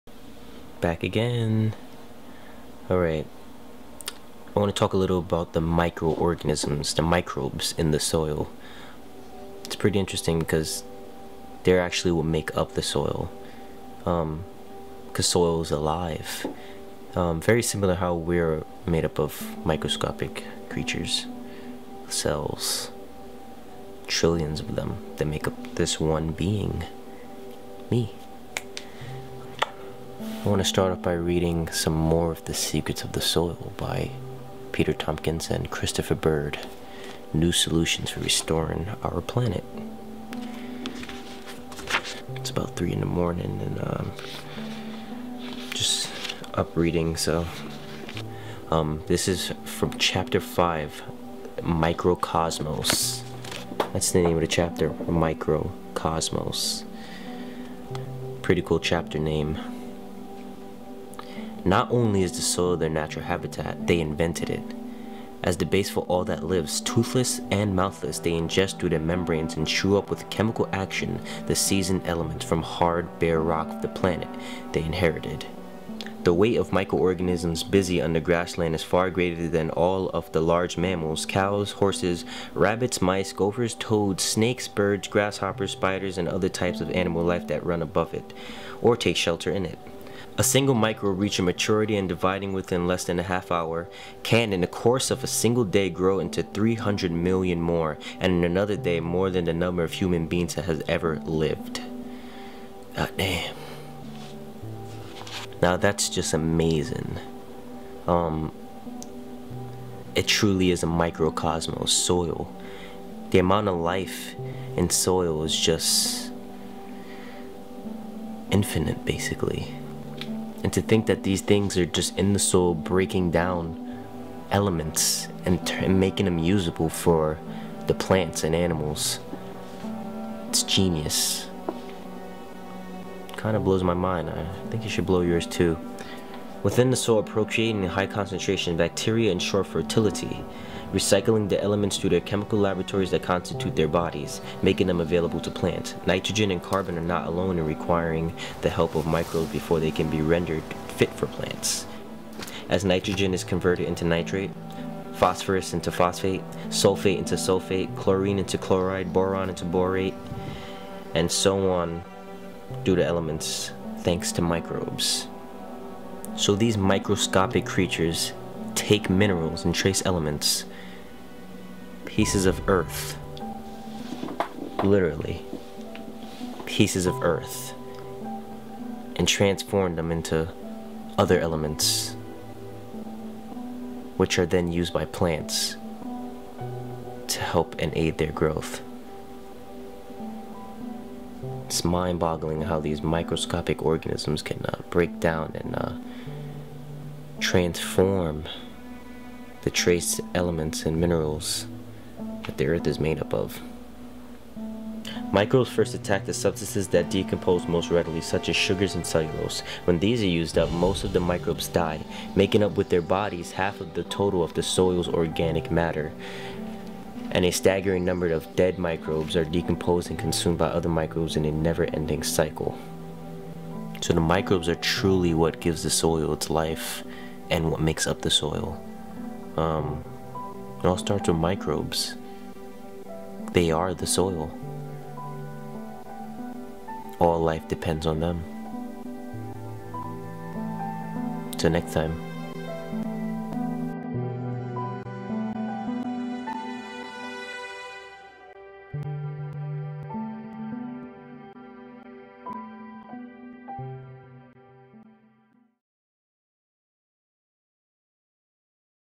Microcosmos in the soil
The soil truly is a Microcosmos. In the soil there is a world of microorganisms who are working to transform trace elements and minerals. They are the soil. The readings are from the book "The Secrets Of The Soil" Chapter 5 "Microcosmos".